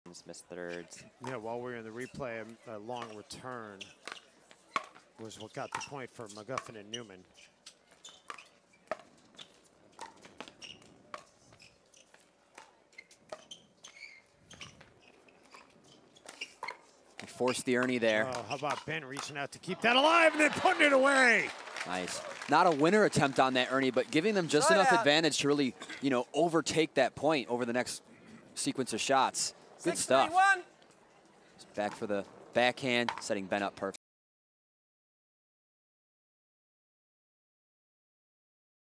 Teamwork Pickleball Highlight - PPA Takeya Showcase
Some of the best duos in the world play as one unit, and that's exactly what Ben Johns and Matt Wright did in this highlight 🤝